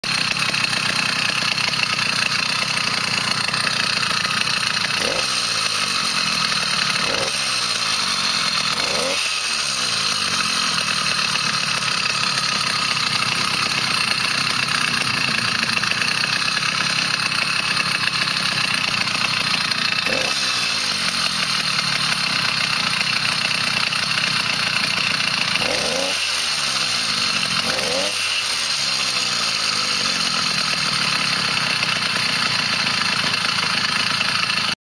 LD100 test
Testing the twin plug ignition systems